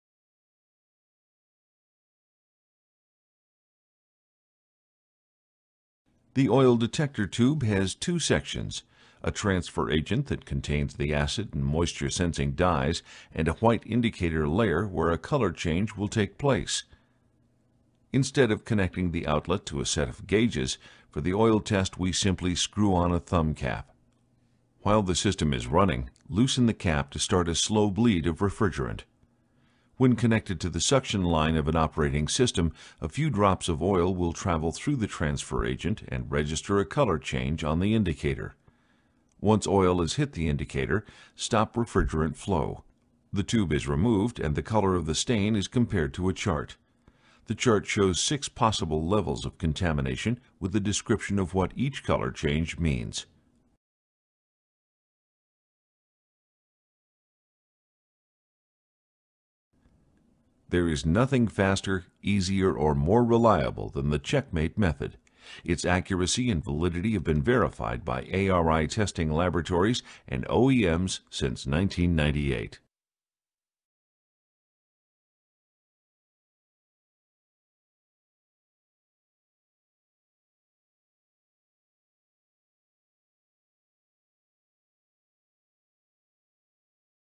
How to use: Refrigeration Technologies Viper Checkmate Acid and Moisture Test
This video shows you how to use Viper Checkmate Kit (Part Number: RT700K)

The Checkmate method is the industry’s only valid field test for measuring the acid and moisture levels (purity) of compressor oil and refrigerant. Predict and prevent the likelihood of compressor failure by checking the contaminant levels in the compressor oil of an operating HVACR system. The Detector Tubes have been tested for accuracy by ETL, Intertek Testing Services, an AHRI-approved laboratory.